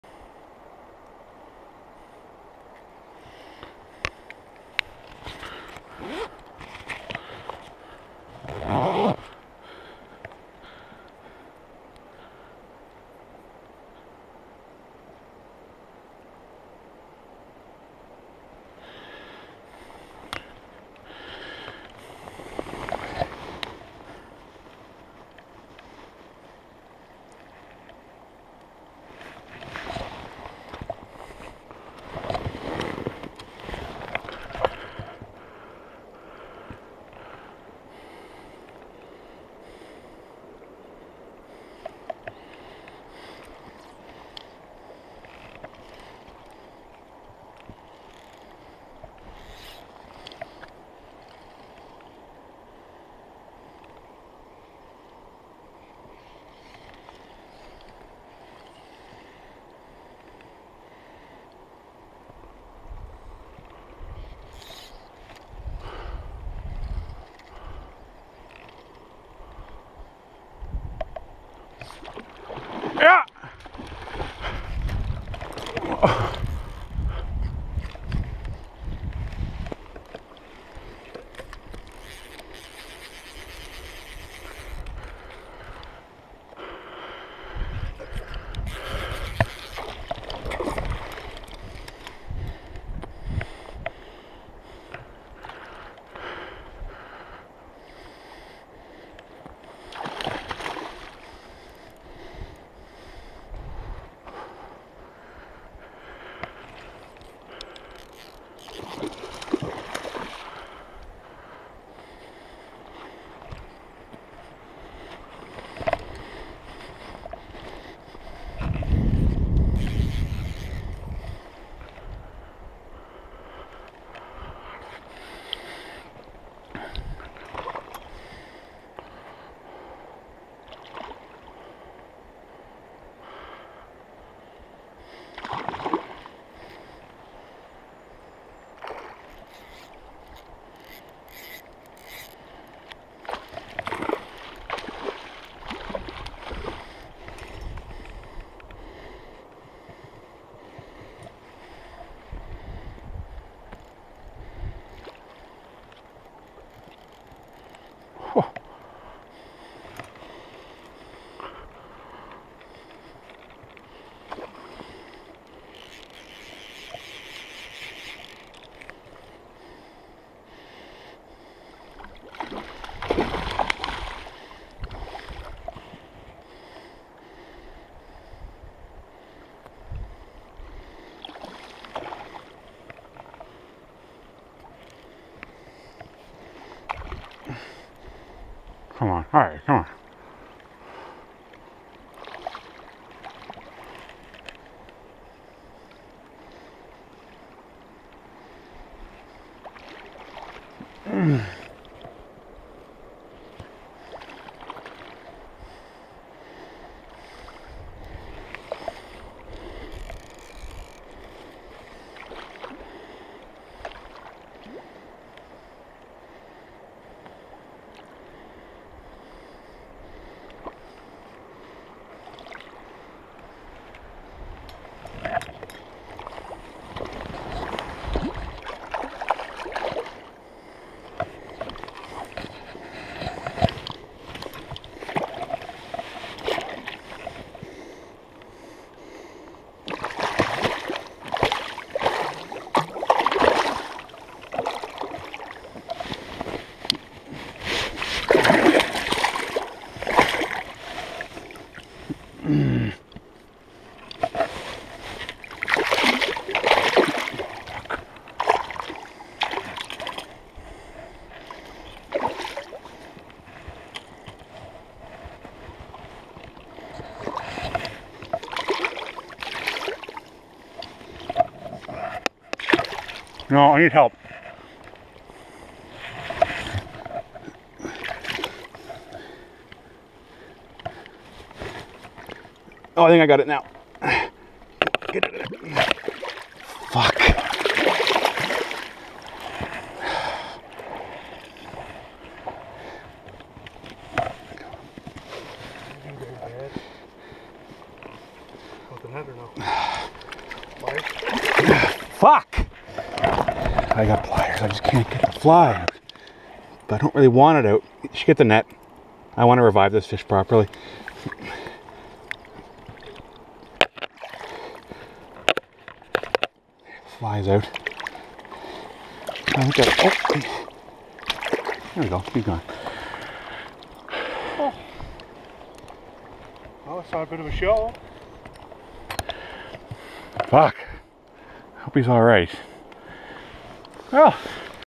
Grilse on Bomber - LSW Miramichi July 4, 2023
Grilse caught on the Little Southwest Miramichi, July 4, 2023. Fly - natural bomber